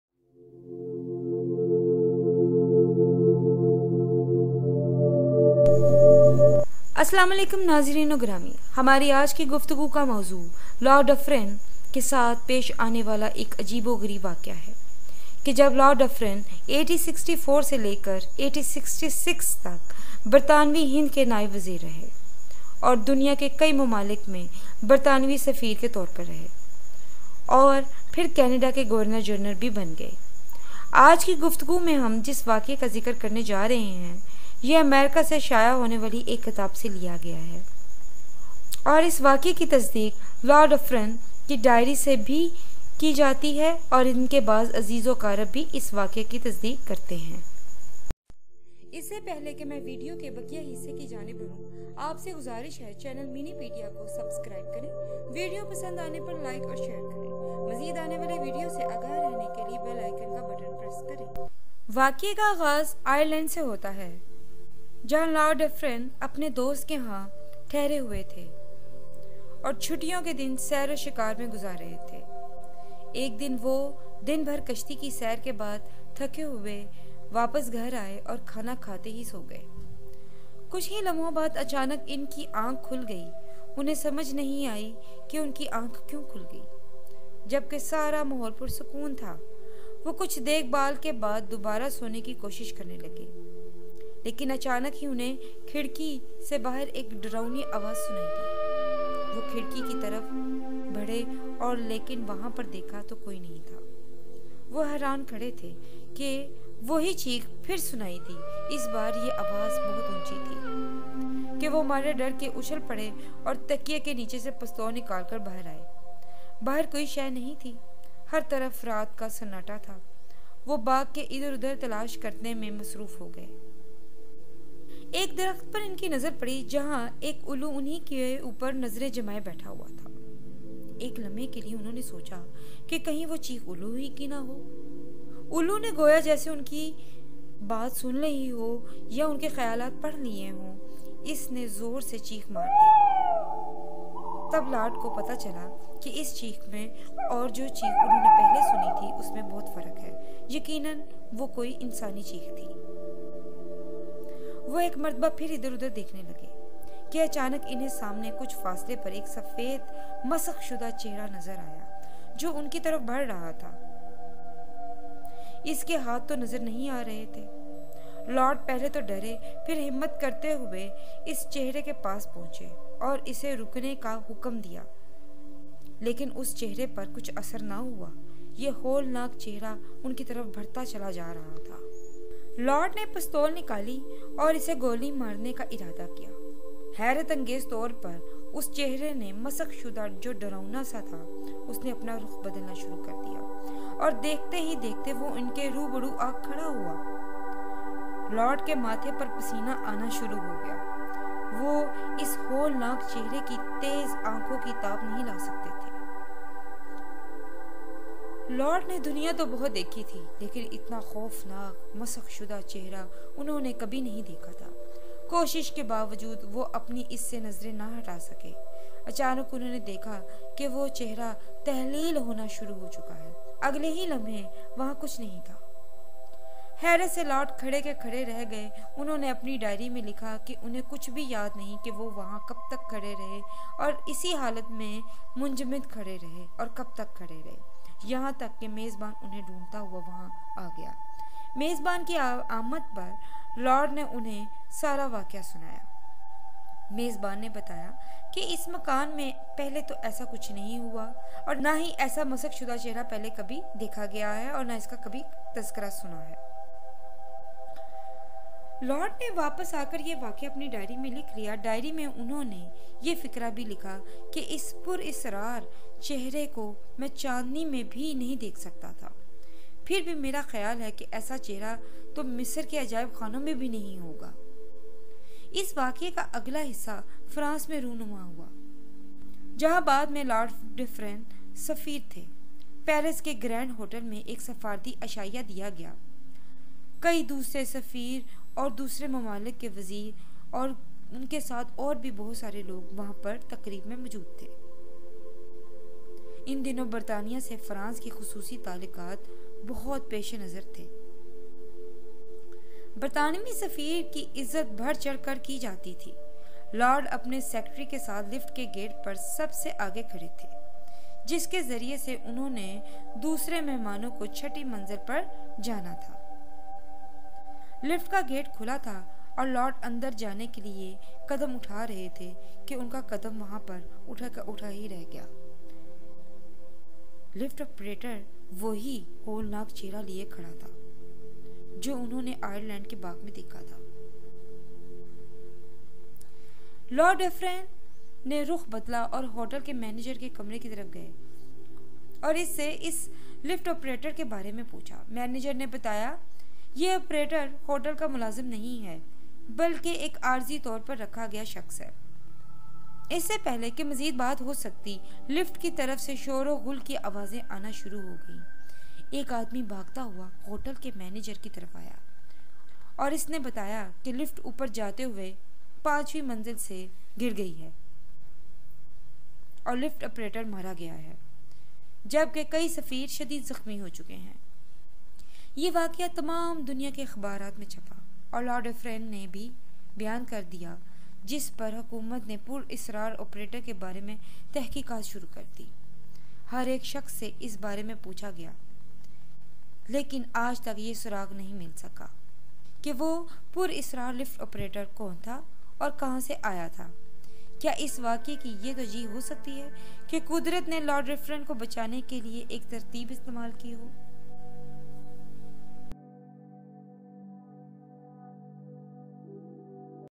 Lord Dufferin's story: Unsolved Mystery of the World
The video is about Lord Dufferin's story: Unsolved Mystery of the World. Did he encounter a ghost?